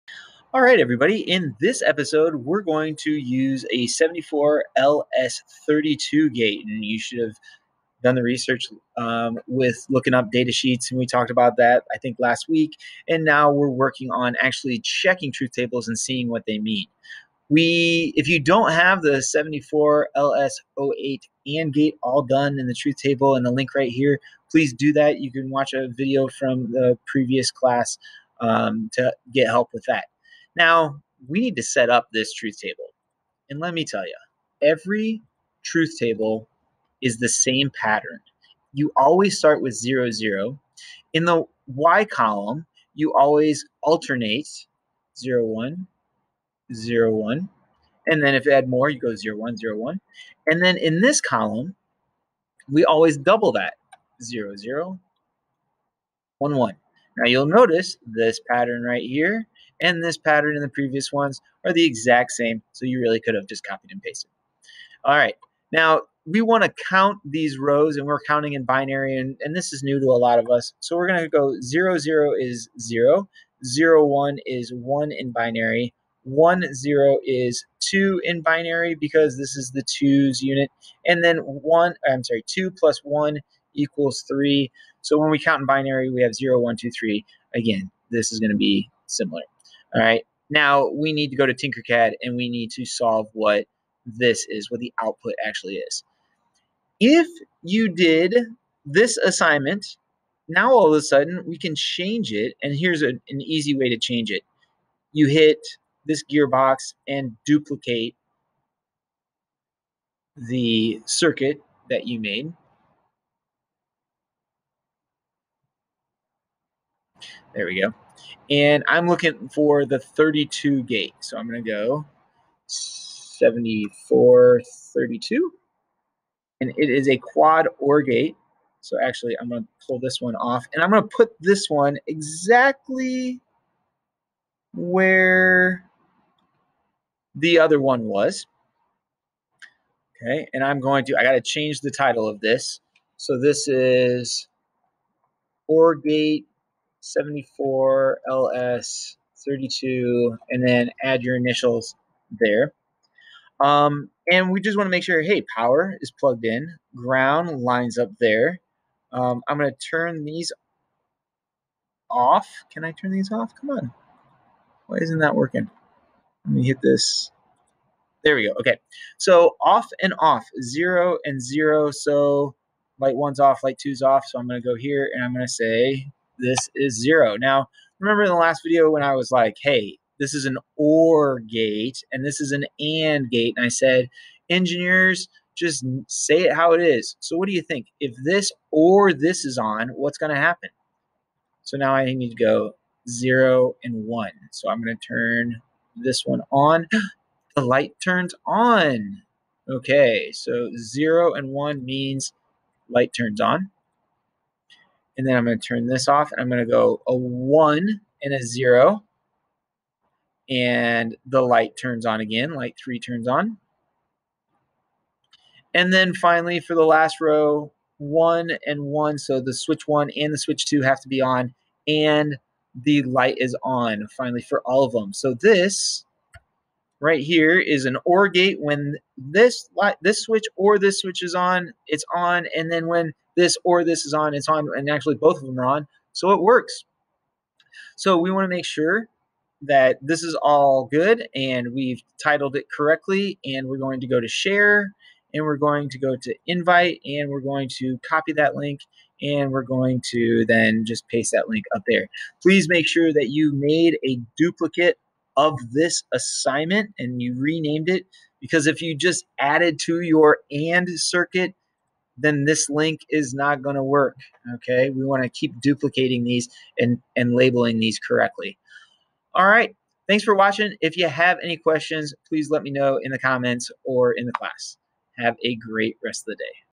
74LS32 OR Gate on TinkerCad
This video shows you how to start using the breadboard with basic logic. We will add an OR gate and complete a truth table.